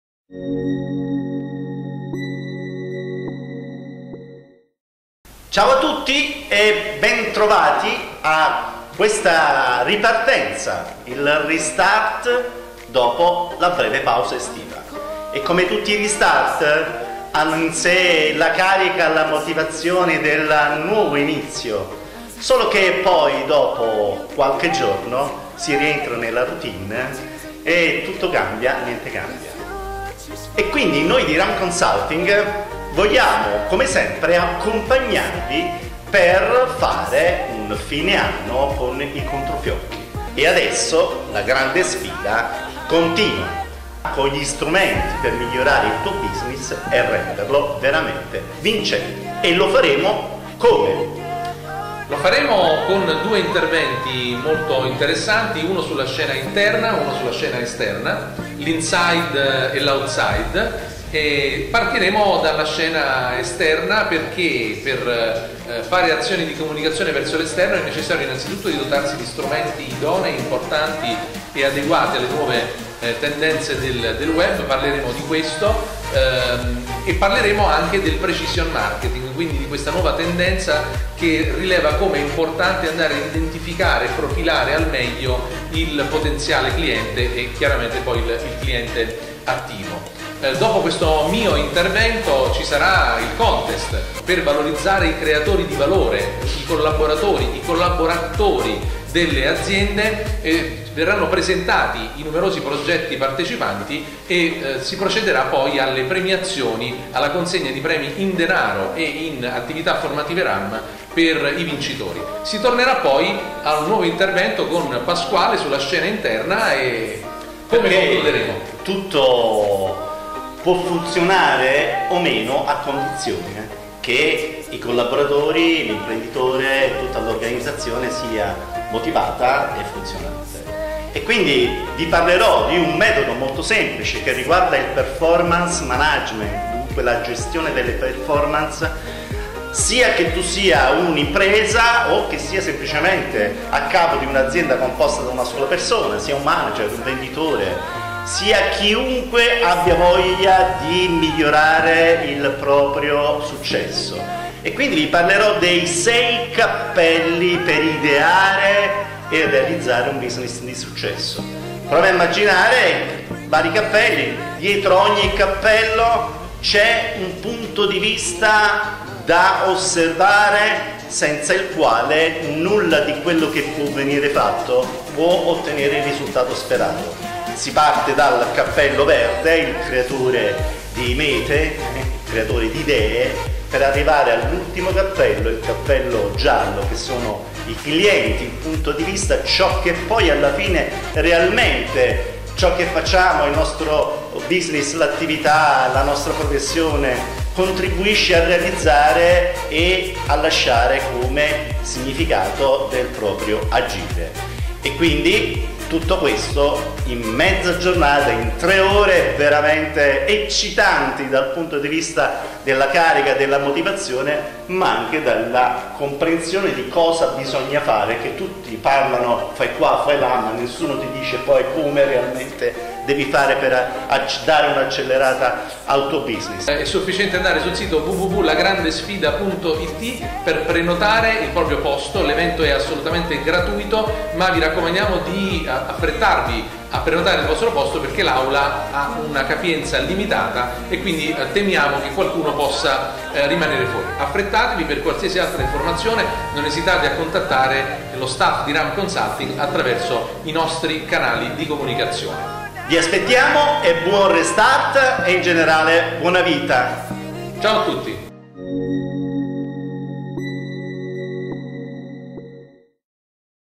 La Grande Sfida...Reloaded 2019
Pasquale Tardino e Dante Ruscello, Coach e Direttore Marketing Ram Consulting, presentano il format dell'evento gratuito La Grande Sfida...Realoded che si svolgerà presso il Work Center Benevento.